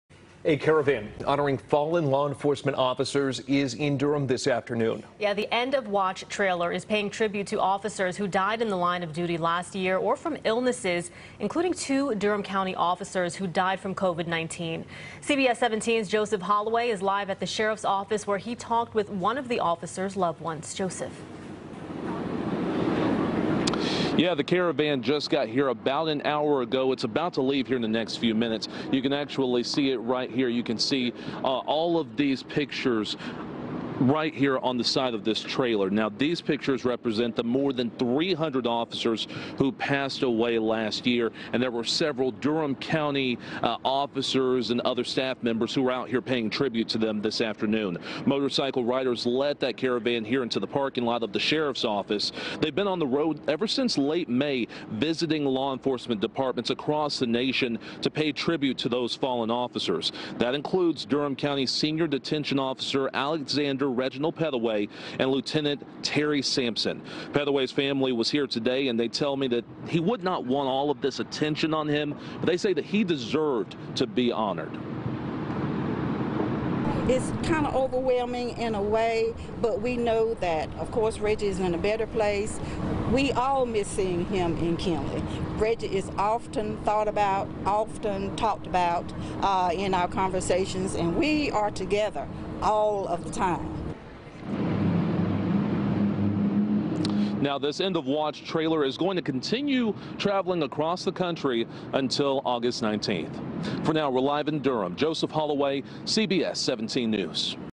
Caravan honoring fallen officers makes stop in Durham
The nationwide "Beyond the Call of Duty - Ride to Remember 2020" caravan made a stop in the Triangle on Friday morning.